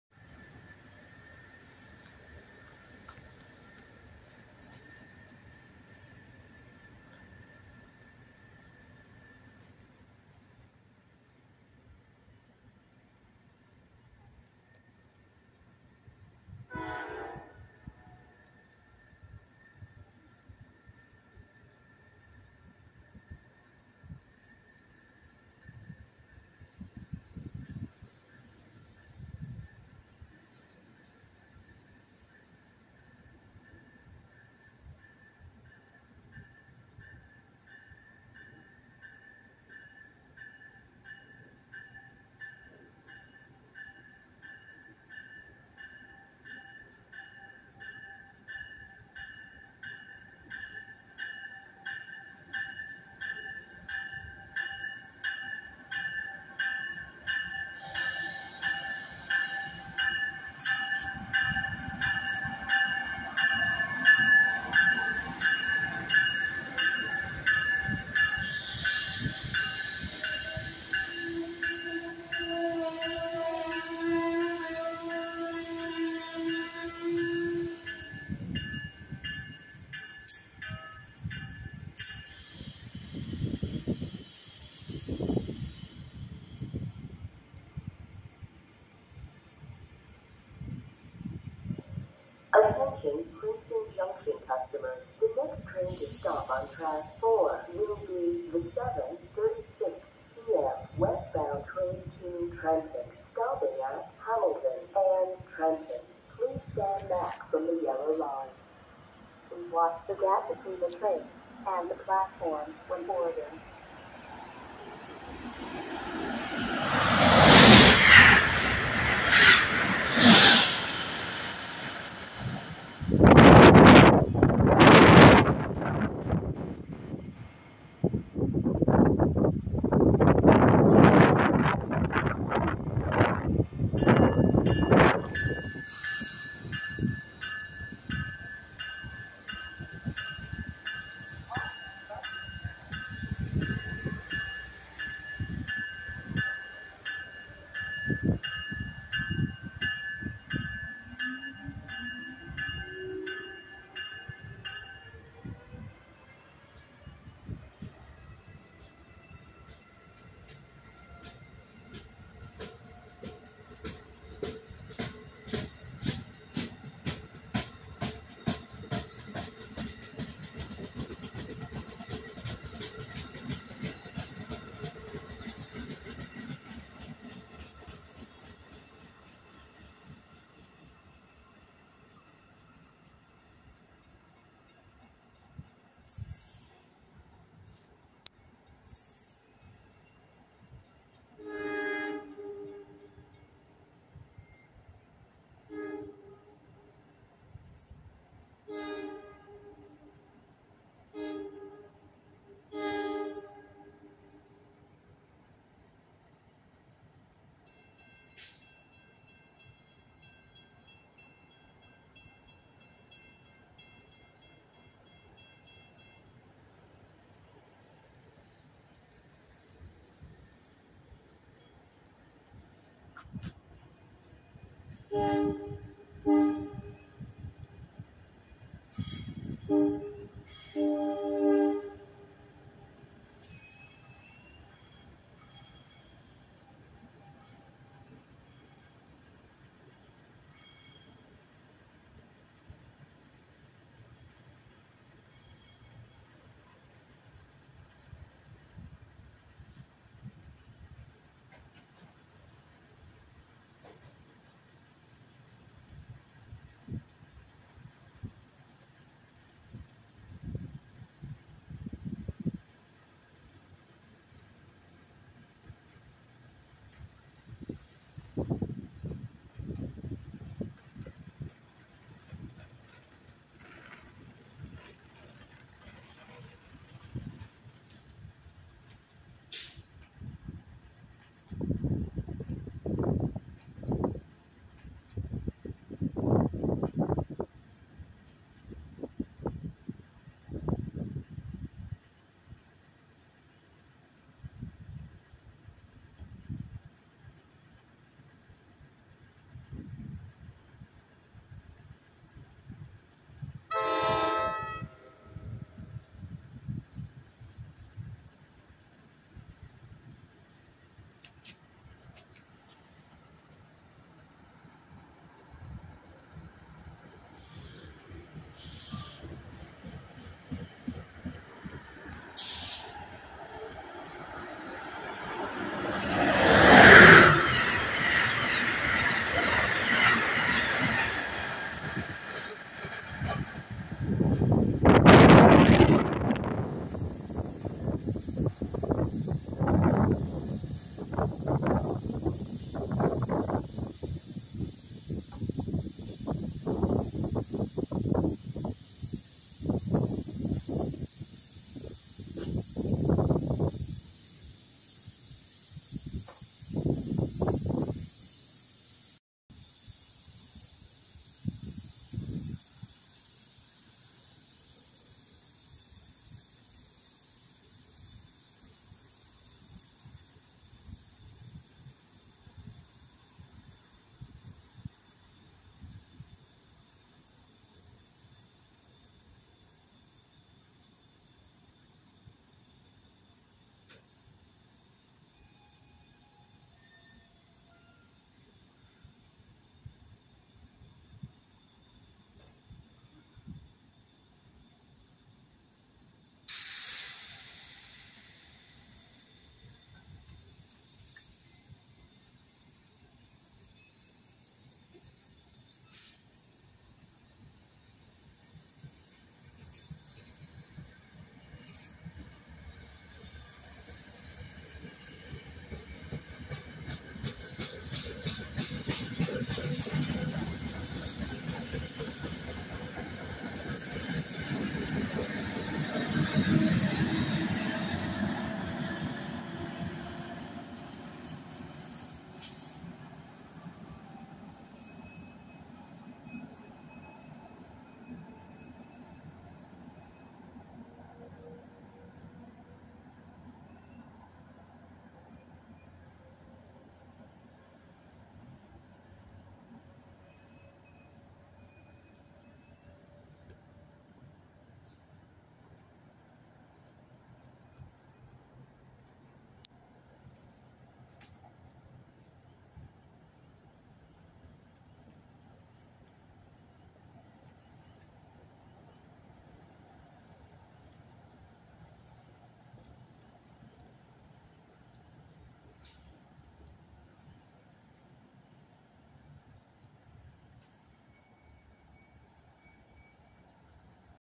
Amtrak & NJ Transit at Princeton Junction March 5th
Amtrak 55 the Vermonter to Washington D.C., Acela Express 2122 to New York, NJ Transit Train 3963 to Trenton with Comets & Amtrak Northeast Regional 136 to Springfield, MA on the Northeast Corridor at Princeton Junction Station on the evening of Tuesday March 5th. 55 appeared in the distance right after I got onto the platform from the lot 🅿️ & apparently now stops here which was quite strange since it always flew past before so I wonder when had it happened, any help is greatly appreciated.